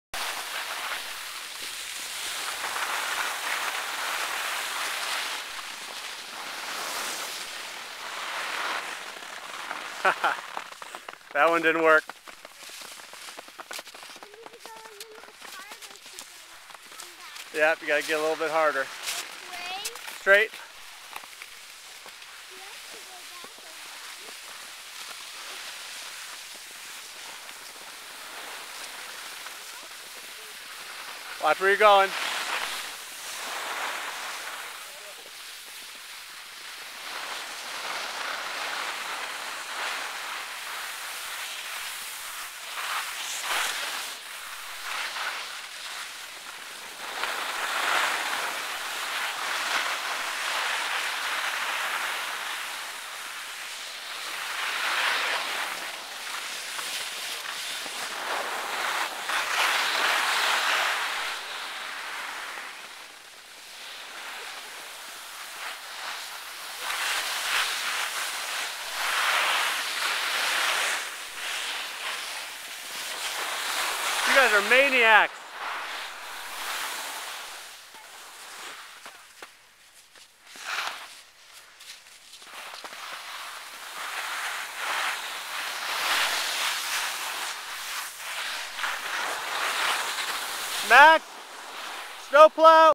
Clio and Max in Champagny
The two play cat and mouse and explore a little "off-piste" action